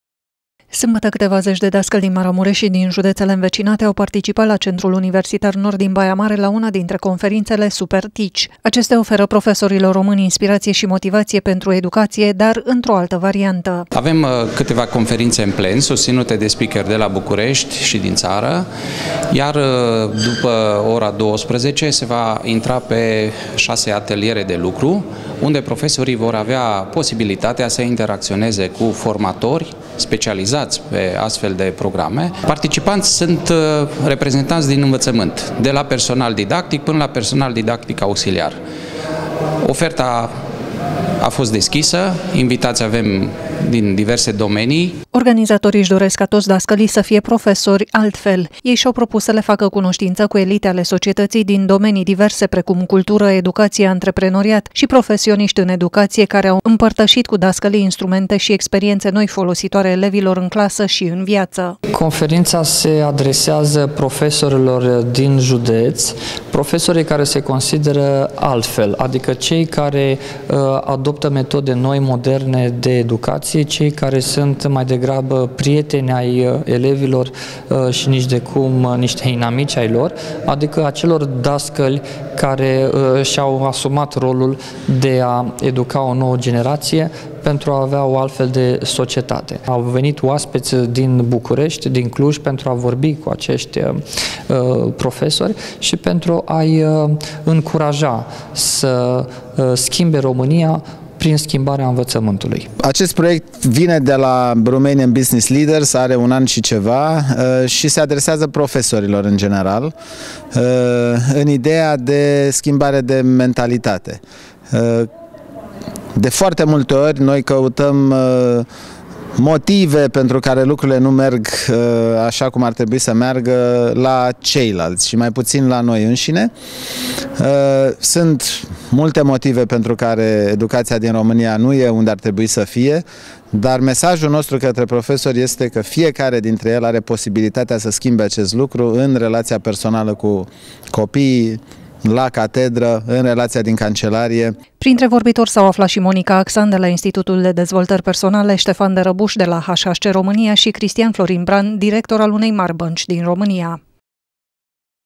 Altfel de profesori pentru un altfel de învățământ
Profesorii din Maramureș au fost ”elevi” la sfârșitul săptămânii trecute.
Mai exact cei care au participat la conferința Super Teach, la care au avut posibilitatea să învețe să fie altfel de dascăli.